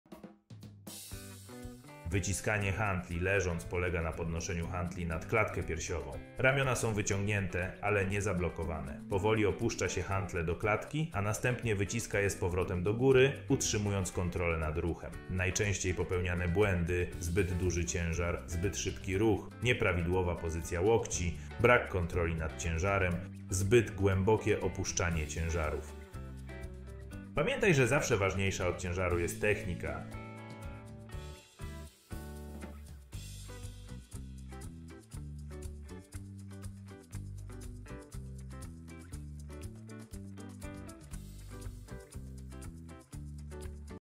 Wyciskanie leżąc z hantlami - Atlas Ćwiczeń DWS 02
Wyciskanie leżąc z hantlami
Wyciskanie hantli leżąc polega na podnoszeniu hantli nad klatkę piersiową  Ramiona są wyciągnięte, ale nie zablokowane. Powoli opuszcza się hantle do klatki z, a następnie wyciska je z powrotem do góry, utrzymując kontrolę nad ruchem.
Najczęściej popełniane błędy:
- Zbyt duży ciężar.
- Zbyt szybki ruch.
- Nieprawidłowa pozycja łokci.
- Brak kontroli nad ciężarem.
- Zbyt głębokie opuszczanie ciężarów.